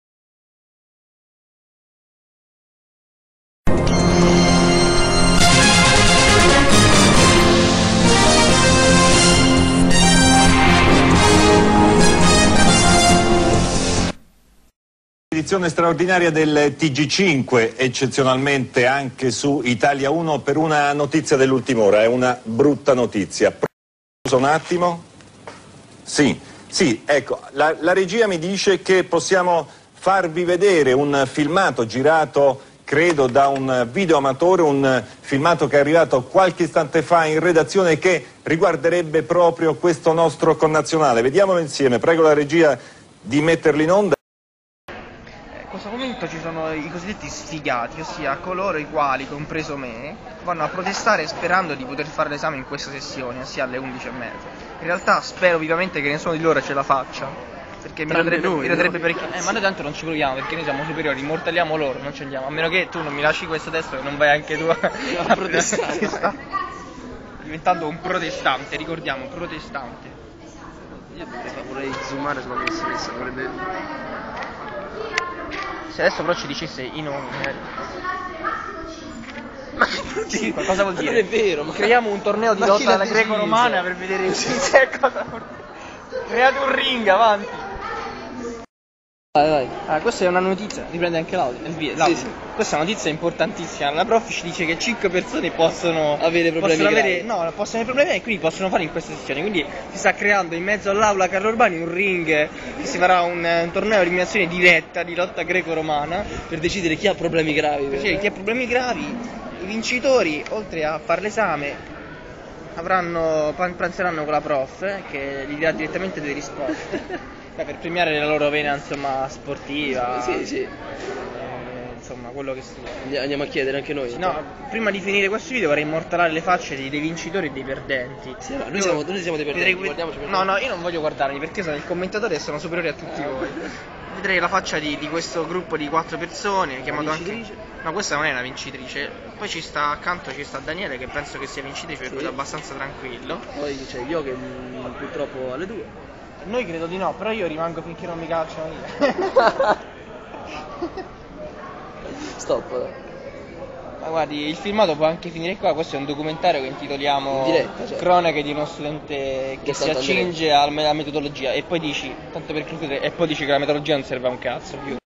come superare l'esame di metodologia
Fantastico video che spiega come superare l'esame di metodologia medica .